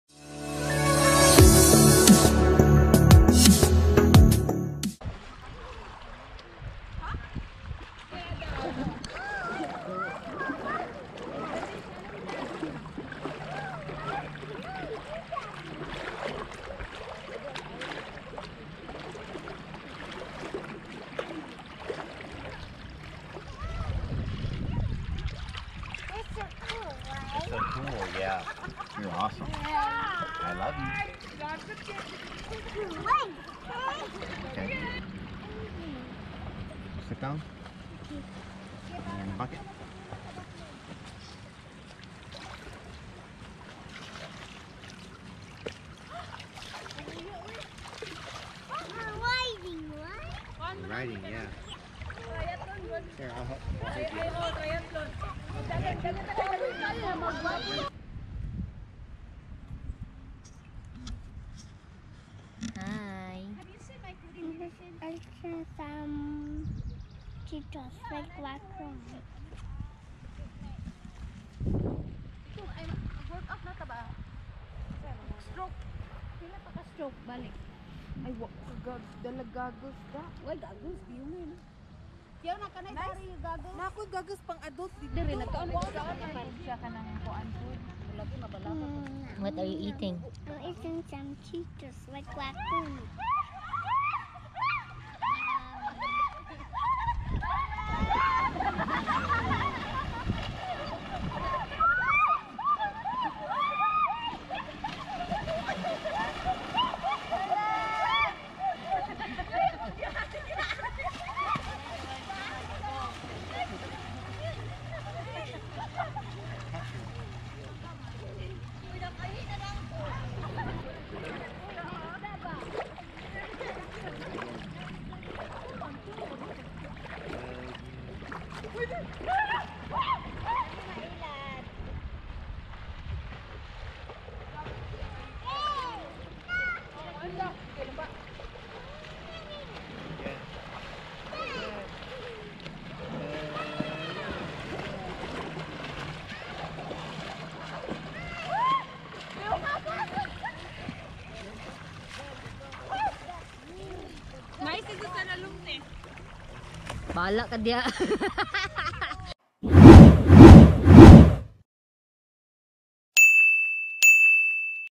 Wisconsin Dells ❗️ Island Pointe Resort | The Maki Vlog
Two amazing nights in this beautiful place!
This year is with our daughter and friends.